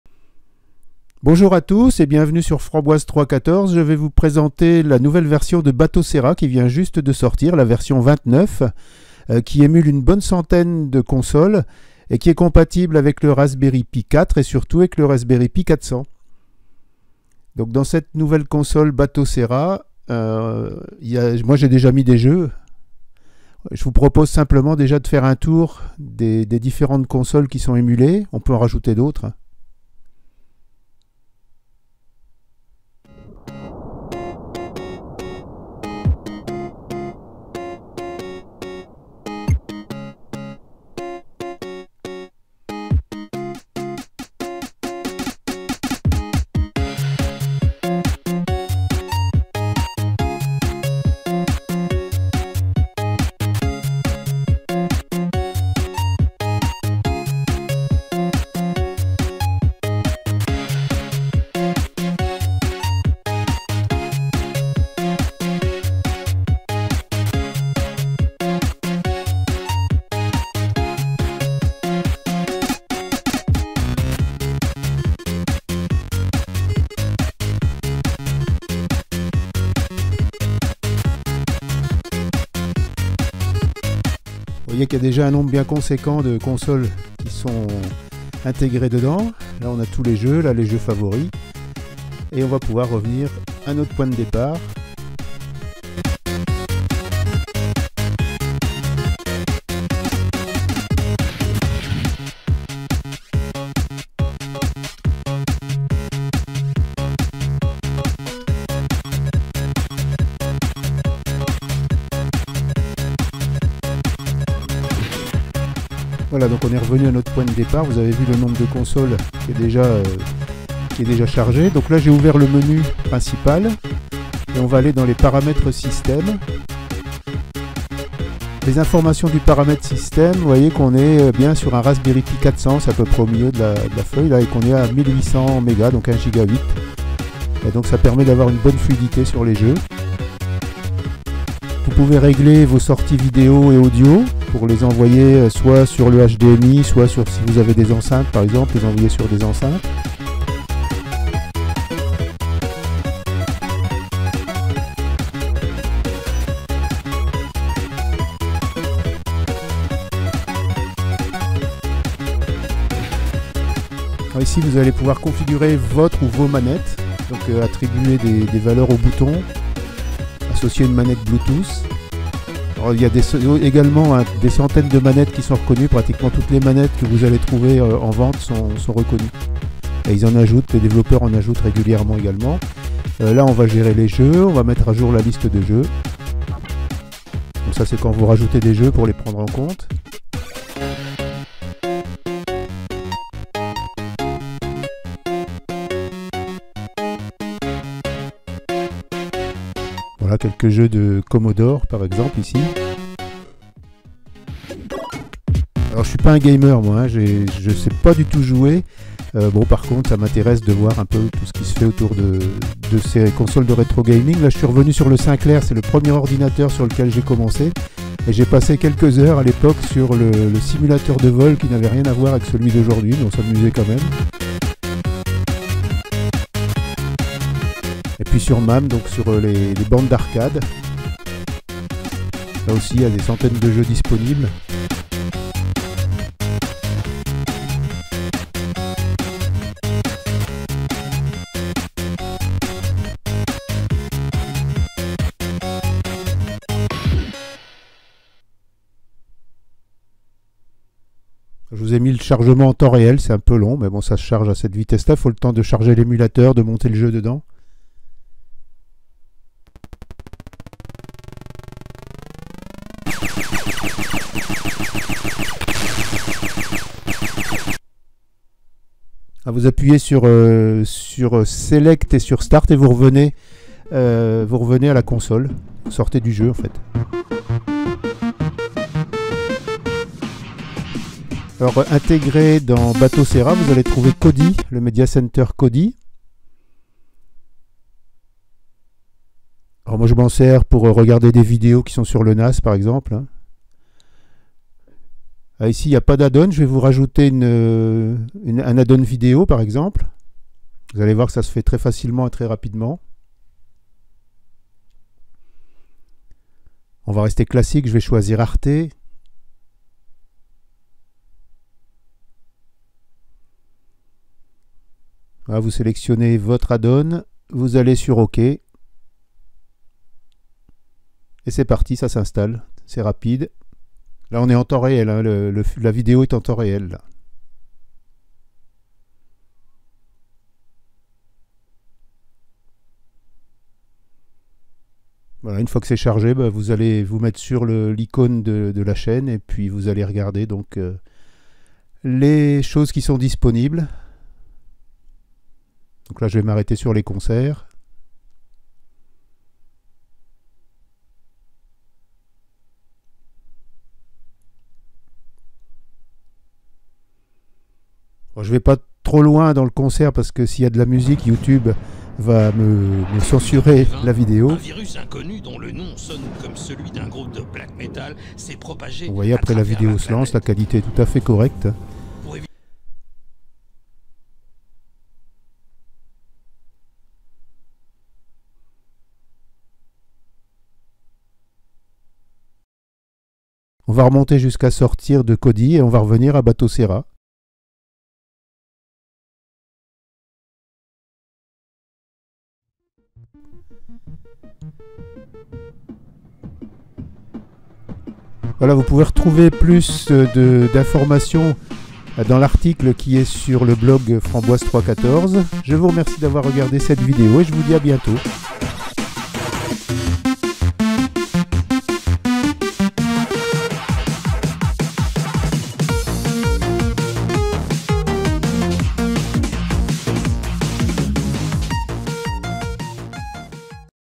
Sortie de la distribution de retrogaming Batocera 29
La version 29 de Batocera est sortie le 11 décembre 2020. Elle émule une centaine de consoles, accepte pratiquement toutes les manettes disponibles sur le marché. Tous vos jeux de console et d'arcade préférés sont à portée de main. La distribution embarque également le mediacenter Kodi, indispensable pour gérer vos vidéos et vos musiques. Pour plus d'informations visitez le blog